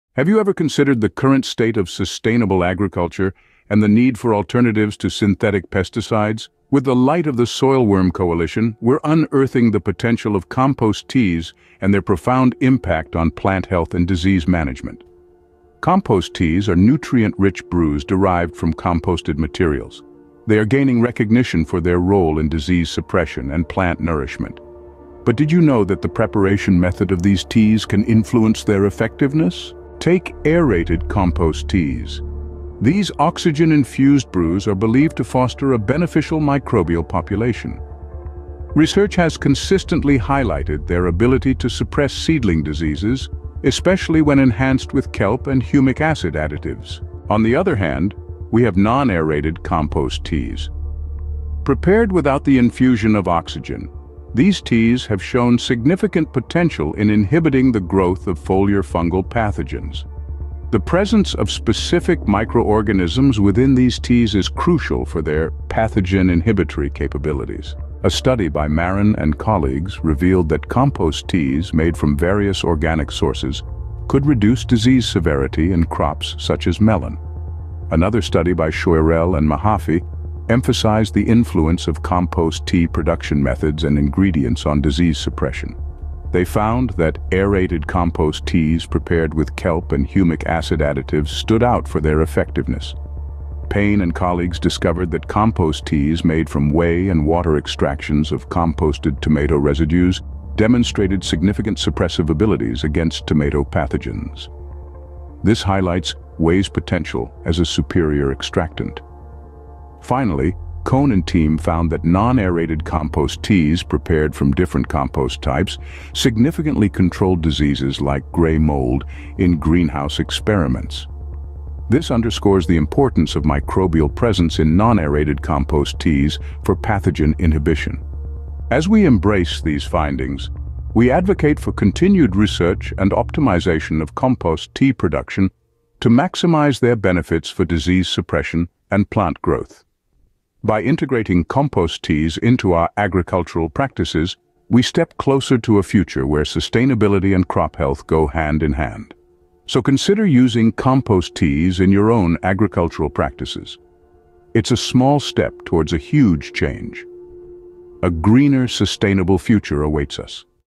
Unlocking the Secrets of Sustainable Agriculture with Compost Teas - with Light of the Soil
Have you ever pondered the future of farming and the quest for eco-friendly alternatives to chemical pesticides? In this enlightening video, we delve deep into the heart of sustainable agriculture, shining a light on the transformative power of compost teas. These nutrient-rich concoctions are not just a fad; they're a pivotal part of the movement towards healthier plants and disease management without the reliance on synthetic solutions.

What You'll Discover:

The Essence of Compost Teas: Explore the world of these potent brews derived from composted organic matter and their rising fame for boosting plant health and fighting diseases.
Aerated vs. Non-Aerated Teas: Learn about the significant impact the preparation method has on the effectiveness of compost teas, from oxygen-rich aerated teas fostering beneficial microbes to non-aerated teas' unique pathogen-fighting properties.
Groundbreaking Research: Dive into the latest findings from leading studies that underscore the disease-suppressive abilities of compost teas, highlighting how ingredients like kelp, humic acid, and even whey can enhance these effects.
Practical Applications: Gain insights into how integrating compost teas into agricultural practices can pave the way for a sustainable, healthier future in farming.
🌍 Join the Movement Towards a Greener Future:
Embracing compost teas in your agricultural endeavors is more than a practice; it's a step towards a monumental shift in how we nurture our crops and the planet. Whether you're a seasoned farmer, a gardening enthusiast, or simply curious about sustainable agriculture, this video is your gateway to understanding how small changes can lead to significant impacts.

👉 What's Next?
Ready to make a difference? Start by considering compost teas for your gardening needs and join us in fostering a greener, more sustainable future.

Small Step Huge Change